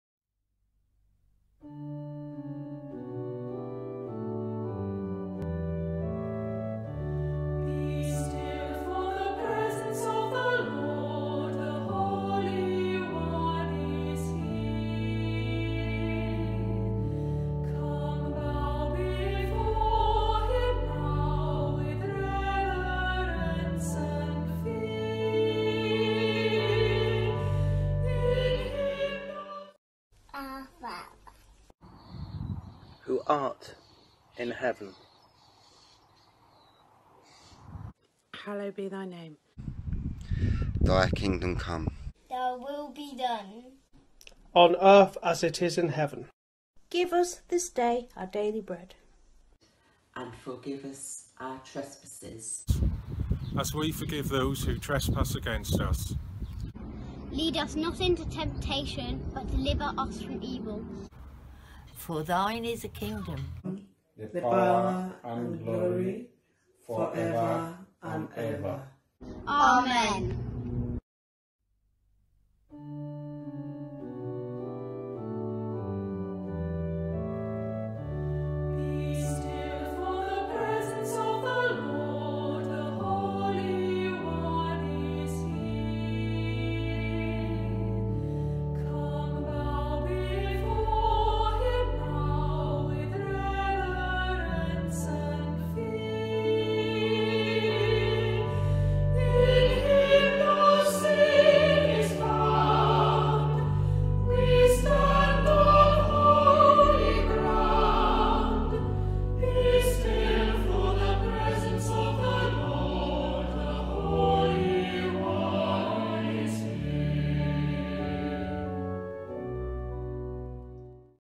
The Lord's Prayer for Pentecost
Every day at noon since Ascension Day, different people from our church family have helped us to pause and pray the Lord's Prayer. Today, for Pentecost they are all joined together.